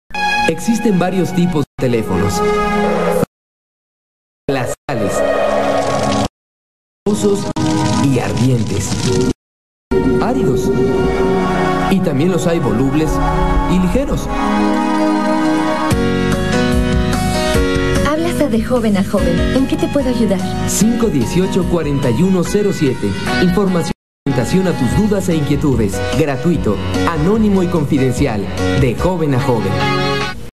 Comerciales mexicanos: CONAPO-DIF-LOCATEL 1998
Servicio telefónico de joven a joven este era otro tipo de servicio aunque se haya creado planificatel pero está en compañia del DIF y LOCATEL